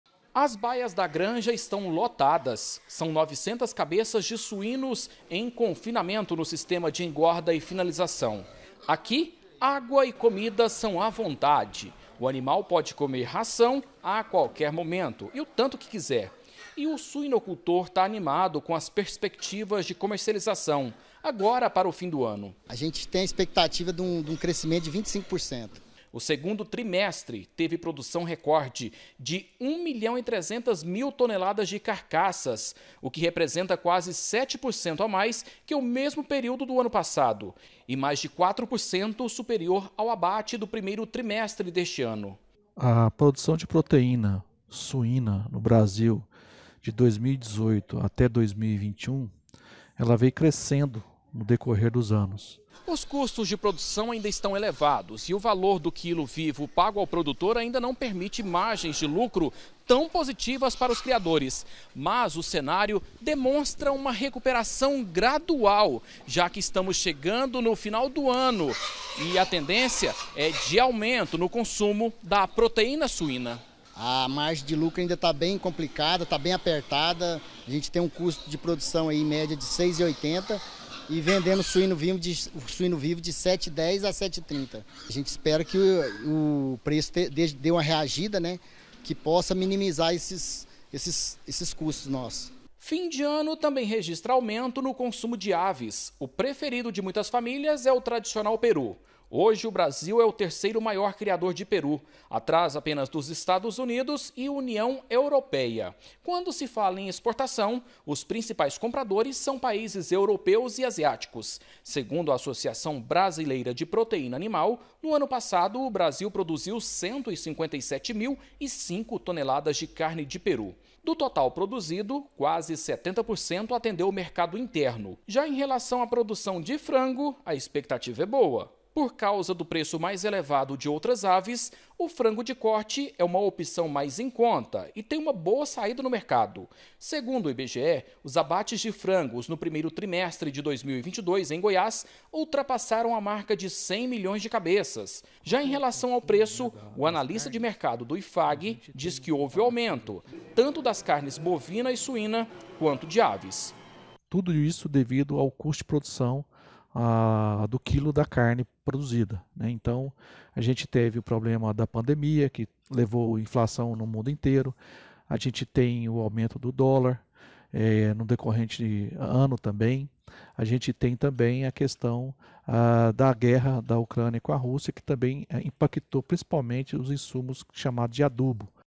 FESTAS: CRESCE CONSUMO DE AVES E SUÍNOS
CRIADORES DE AVES E SUÍNOS ESTÃO ANIMADOS COM O NATAL COMO A CARNE BOVINA CONTINUA CARA, ELES DEVEM VENDER COMO NUNCA.